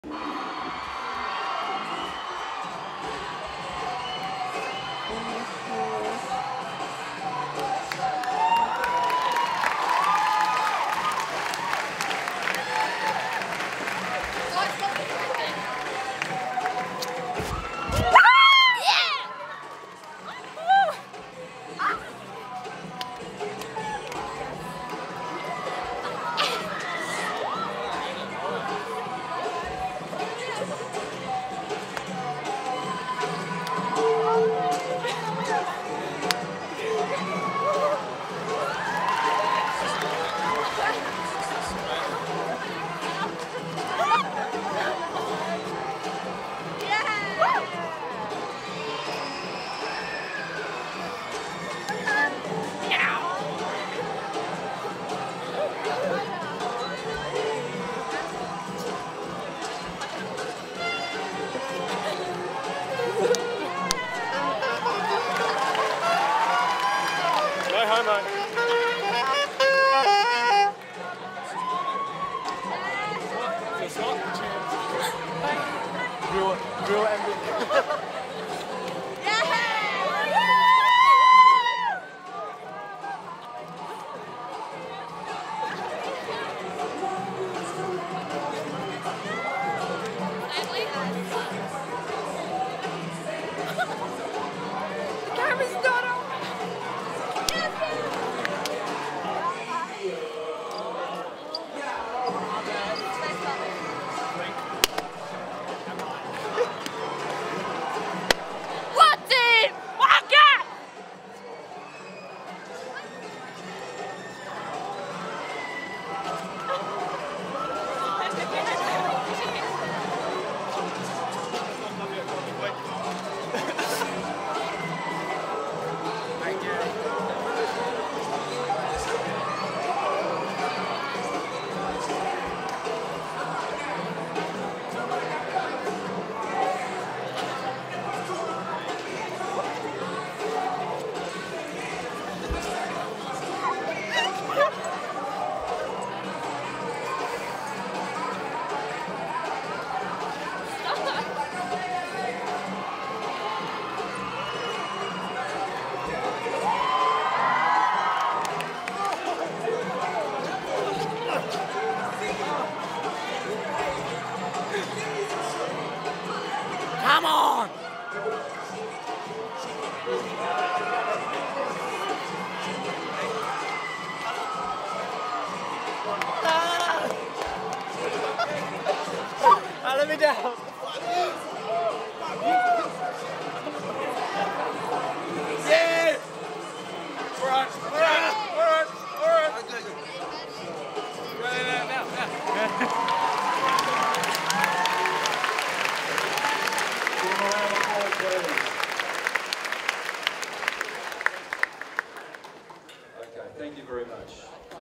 Glenunga year 12's Exit
Glenunga International High School year 12's 'walk out' at final assembly